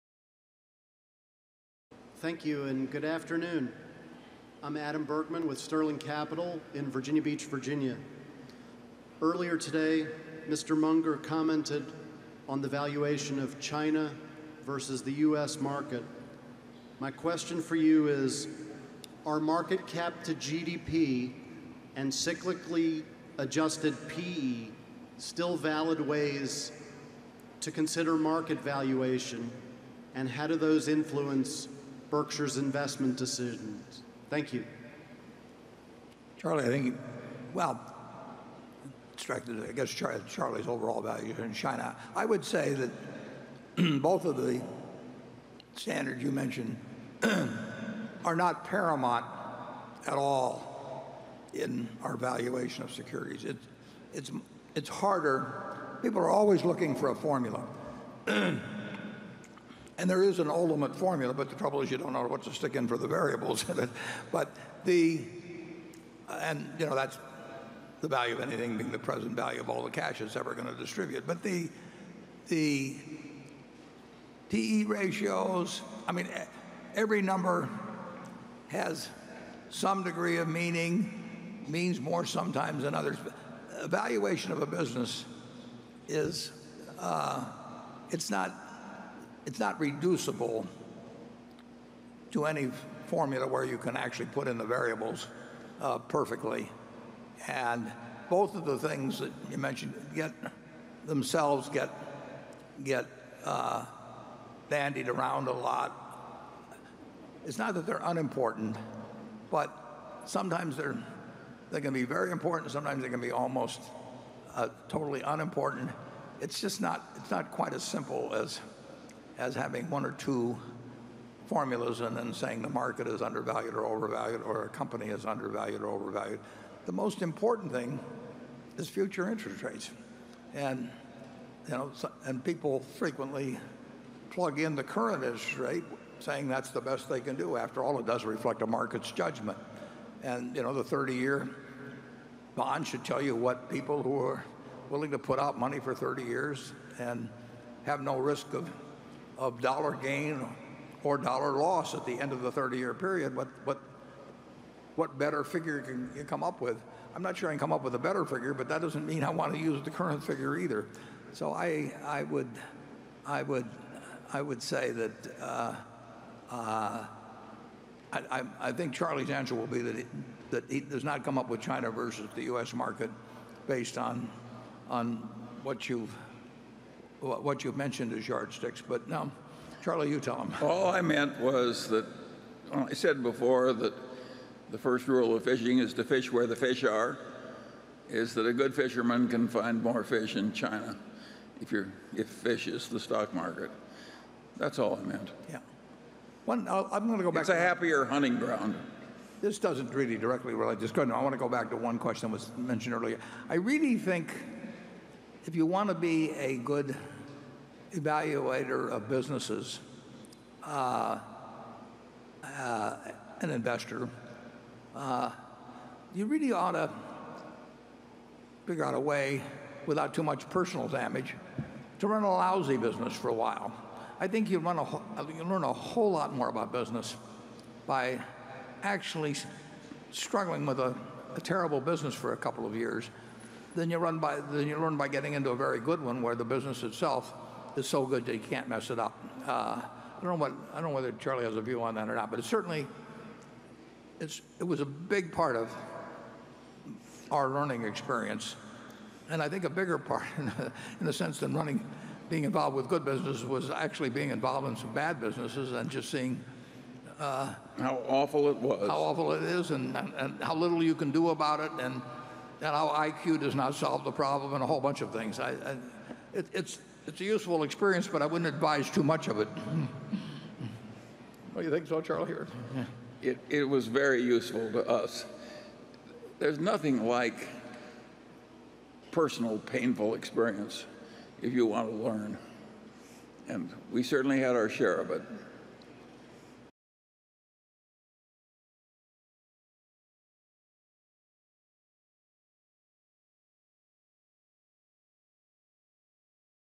Warren Buffett: How I Do Market Valuation in Investment
Buffett is known as a businessman and philanthropist. But he's probably best known for being one of the world's most successful investors. Which is why it's not surprising that Warren Buffett's investment strategy has reached mythical proportions.

Buffett follows several important rules and an investment philosophy that is widely followed around the globe.

In this video Buffett talks about how he does market valuation in Berkshire Hathaway’s investment. He says both GDP and P/E ratio are not the most important.

Attribution: “2017 Berkshire Hathaway Annual Meeting | Warren Buffett | Charlie Munger” by Buffett Online.

Warning: This video is to show you Buffett’s opinion about market valuation Please do your research before doing any investment. A good balance between return and risk is the key to investment success.